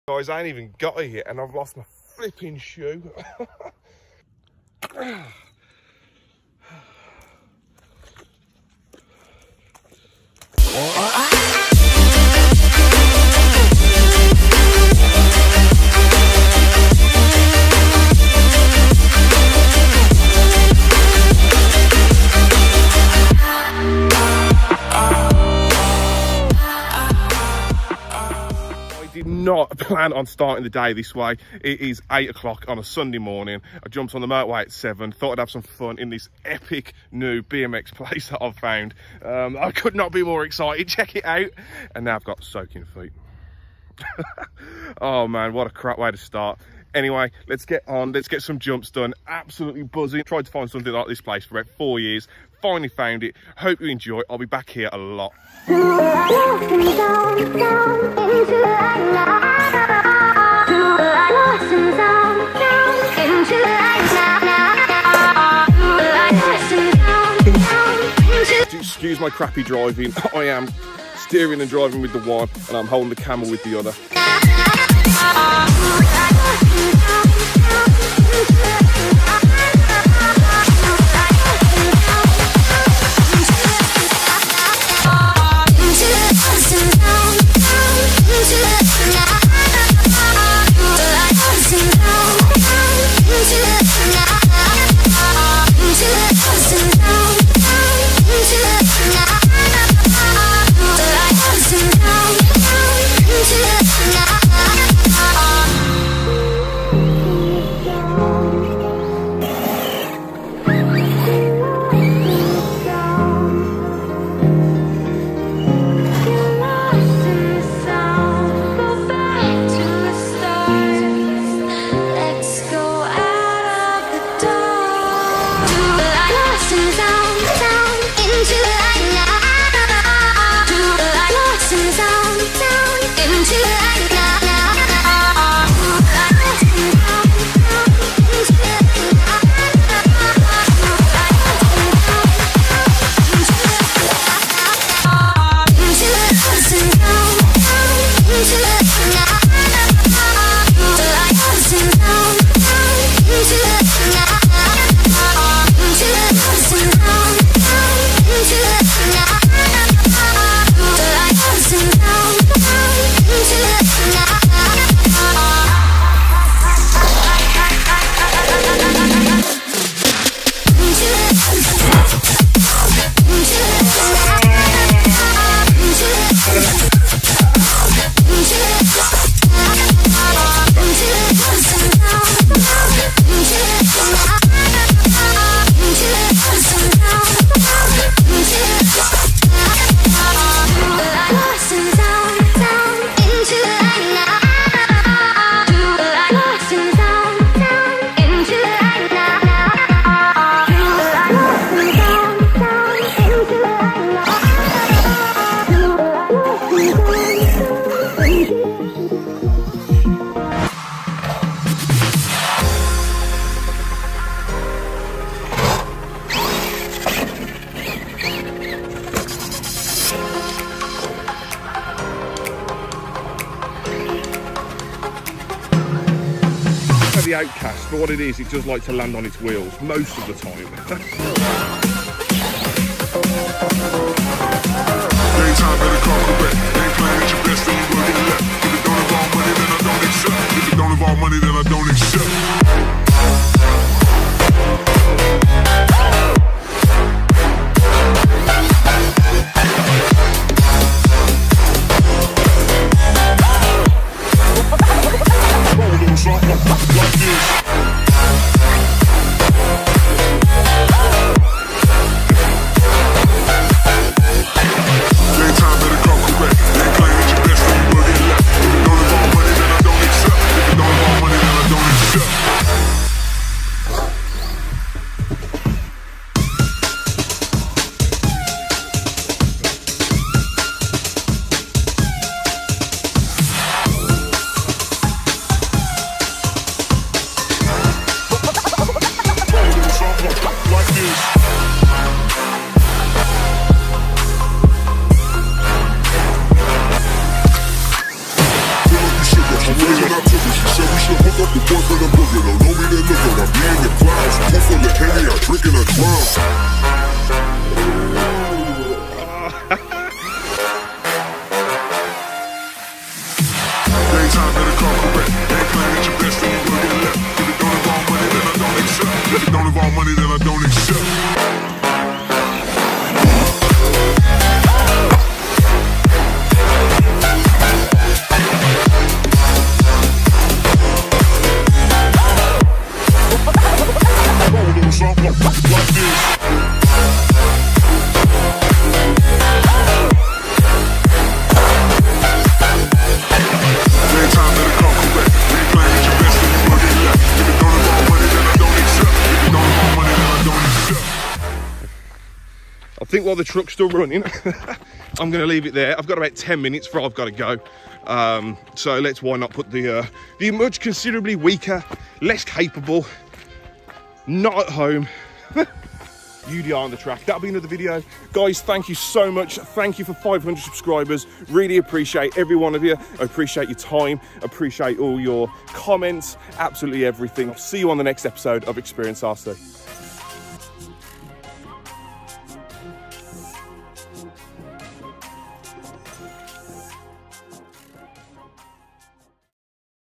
Bashing the Arrma Outcast at my local BMX Track
What a great place to bash my Arrma Outcast!

Want to see the updates before anyone else? and follow!

Want to see Behind The Scenes Footage, then visit my Facebook